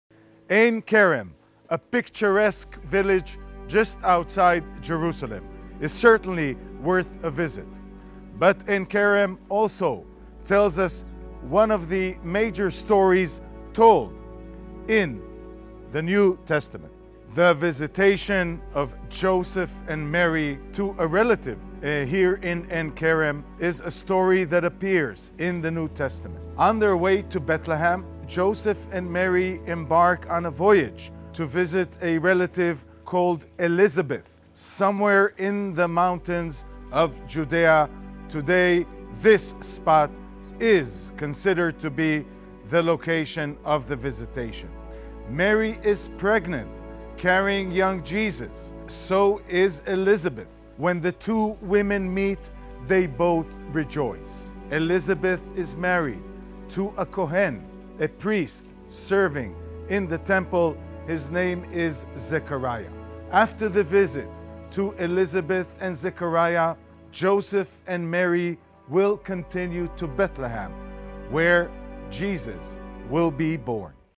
The Mery's Spring (Subscribe and join)
Ein Kerem (Hebrew: עֵין כֶּרֶם‎, ʿEin Kerem lit. "Spring of the Vineyard"; in Arabic ʿAyn Kārim;[9] also Ain Karem, Ein Karem) is an ancient village southwest of historical Jerusalem, and now a neighborhood of the modern city, within Jerusalem District, Israel. It is the site of the Hadassah Medical Center.
Christian tradition holds that Saint John the Baptist was born in Ein Kerem, following the biblical verse in Luke saying John's family lived in a "town in the hill country of Judea".